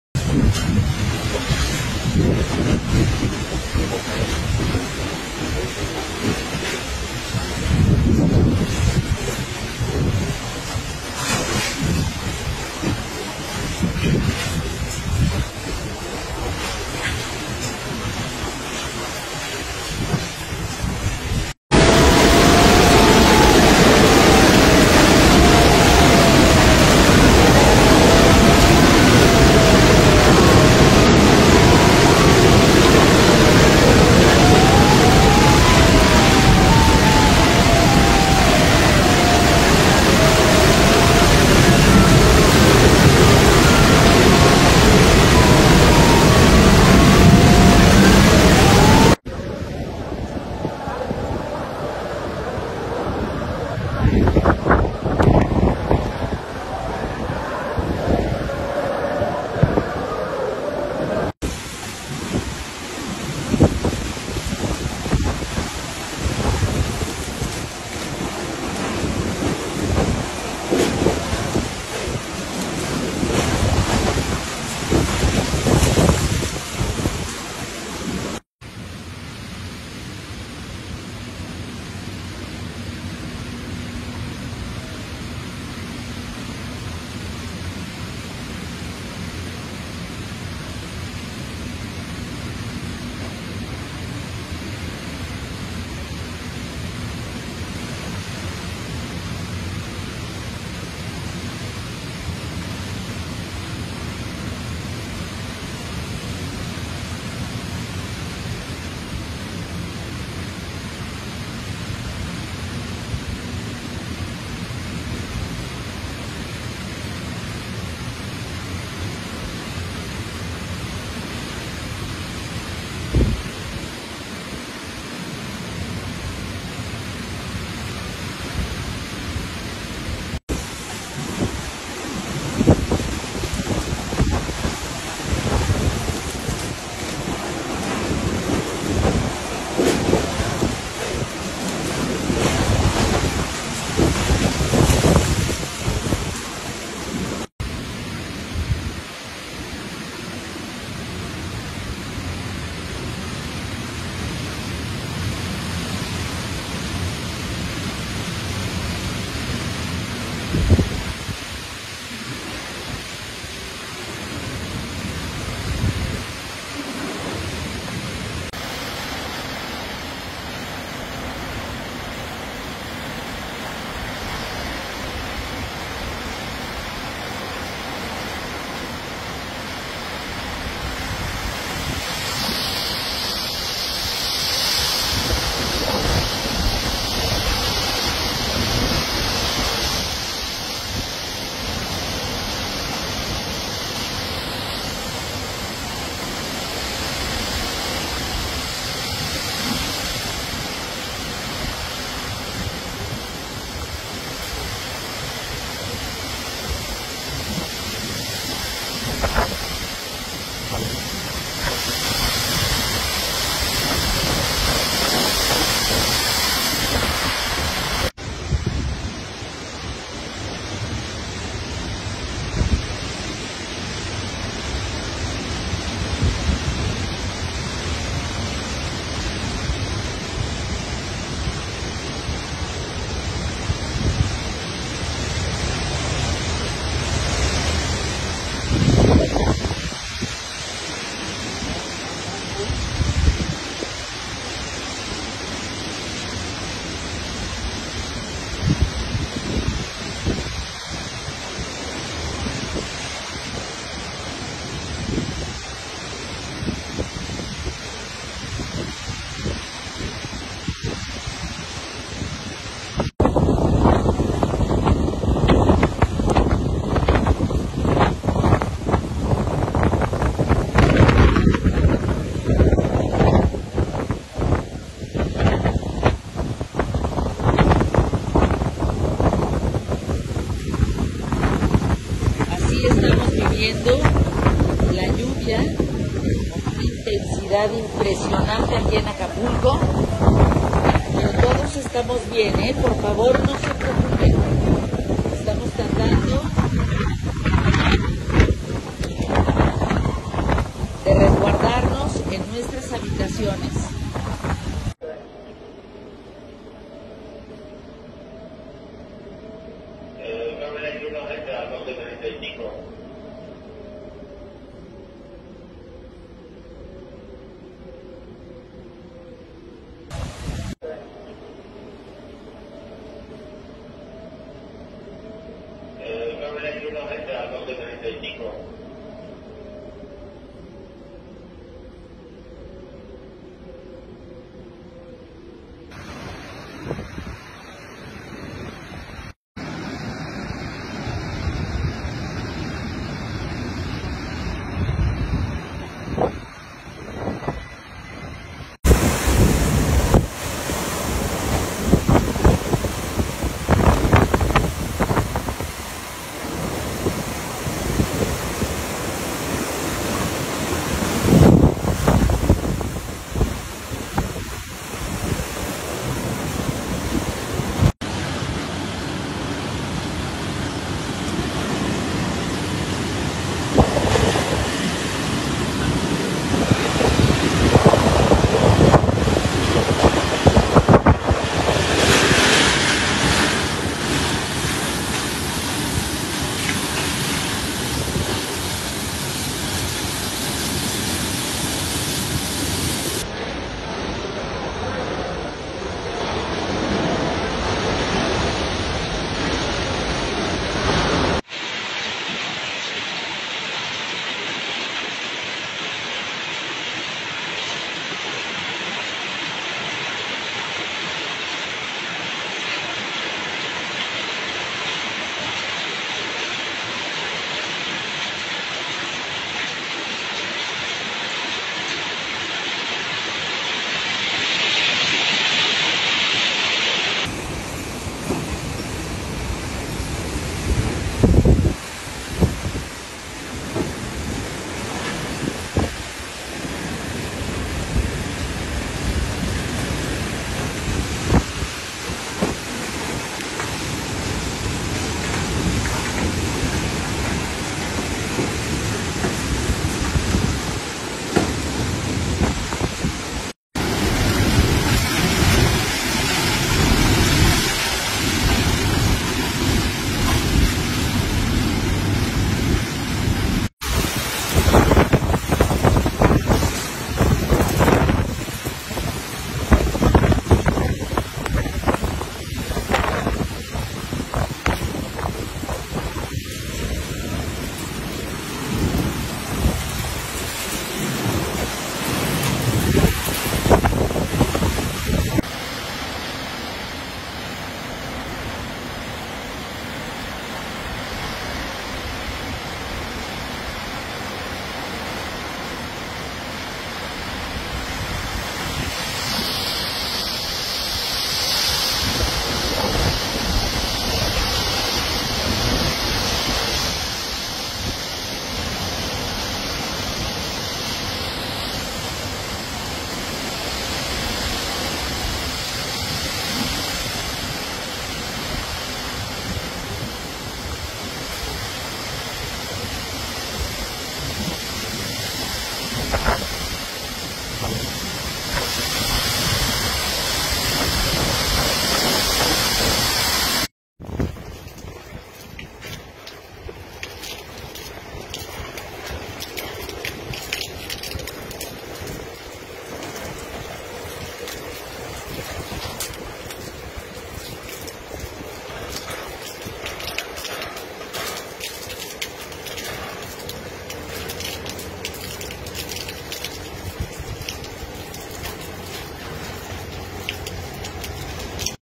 Otis, huracán categoría 5, devasta Acapulco, Guerrero
Algunos datos para entender la situación en Guerrero, tras el paso del Huracán

Vientos sostenidos de 270 kilómetros por hora y tachas de 330 km/h., algo inédito en el Pacífico mexicano.

En 12 horas pasó de ser un moderado meteoro categoría 2 a un peligroso nivel 5 de la escala Saffir Simpson.

En octubre de 1997, el mortífero Huracán Paulina, que azotó Chiapas, Oaxaca y Guerrero, alcanzó una categoría 4.

Se reportan 27 muertes y  cuatro desaparecidos.

La Asociación de Hoteleros del estado de Guerrero reporta severos daños en el 80% de la infraestructura turística.